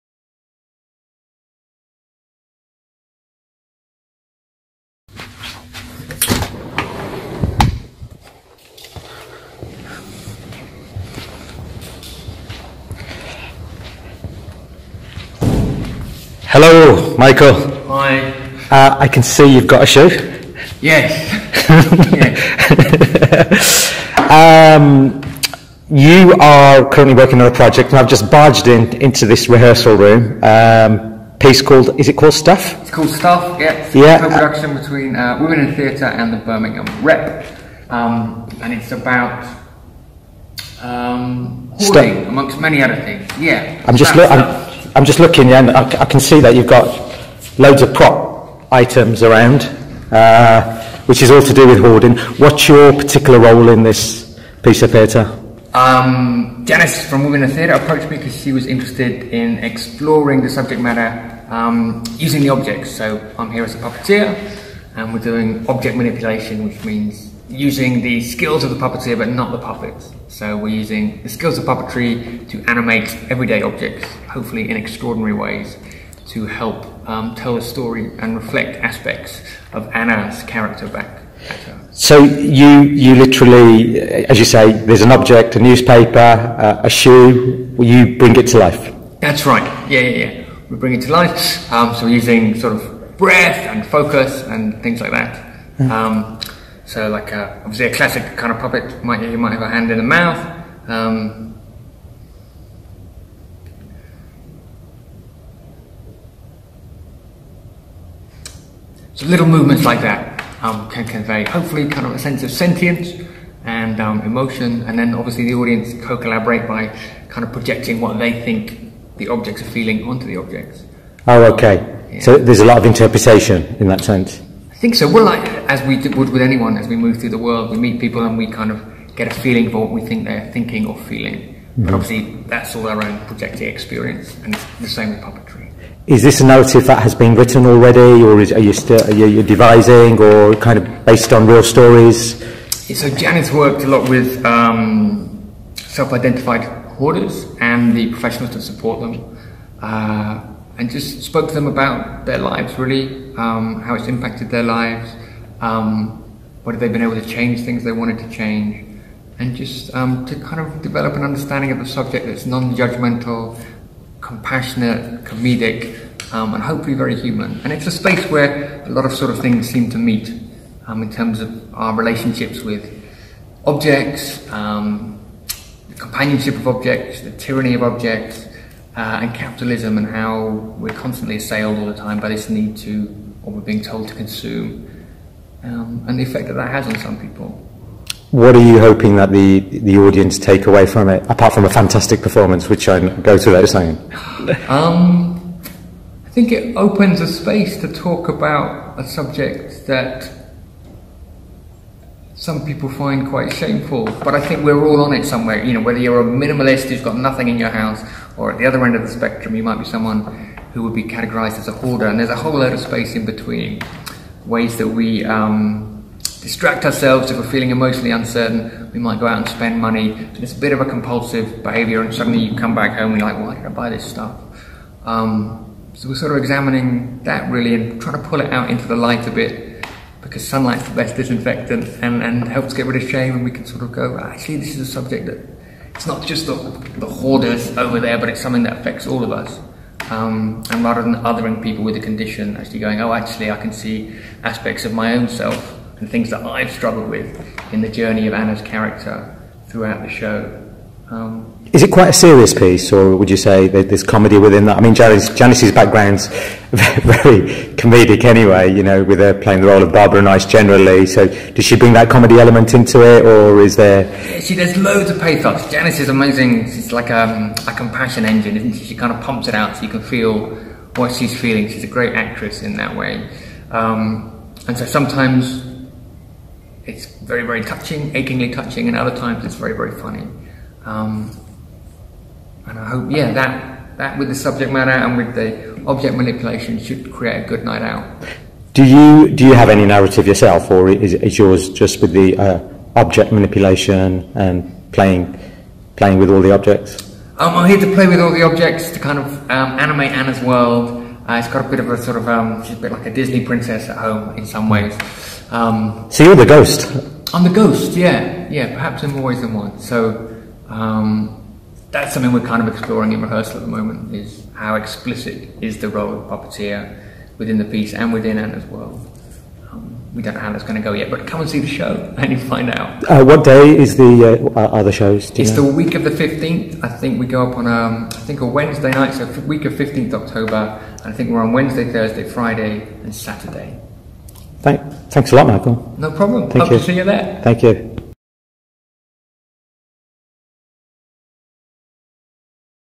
Interview: Puppeteer Michael Crouch on STUFF, a play about hoarding | I Am Birmingham
Vimal Korpal catches up with puppeteer Michael Crouch at the Birmingham Rep Theatre, ahead of his performance with Janice Connolly in the play STUFF, on between 17th and 20th October 2018.